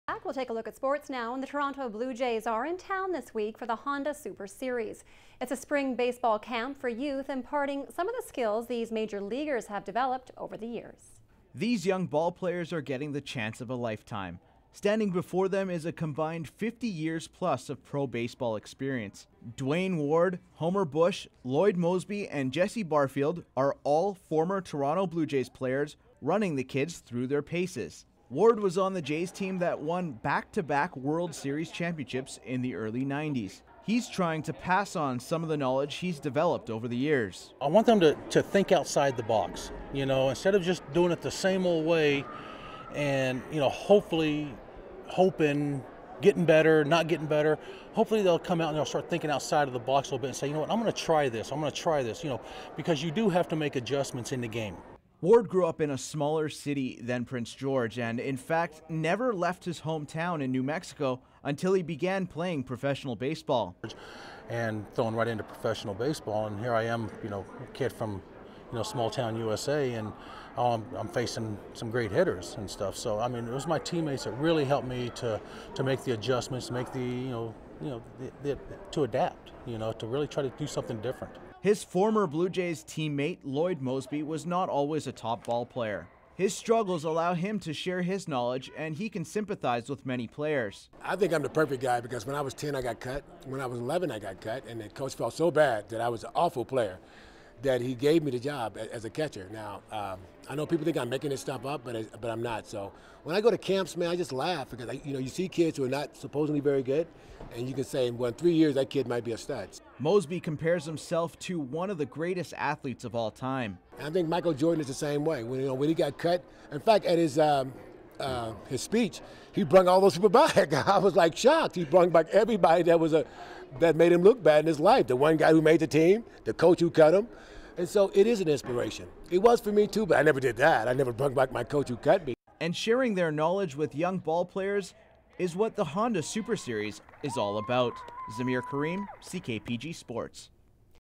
Blue Jays In Town For Training Camp
The Toronto Blue Jays are in town this week for the Honda Super Series. It's a spring baseball camp for youth imparting some of the skills these major leaguers have developed over the years.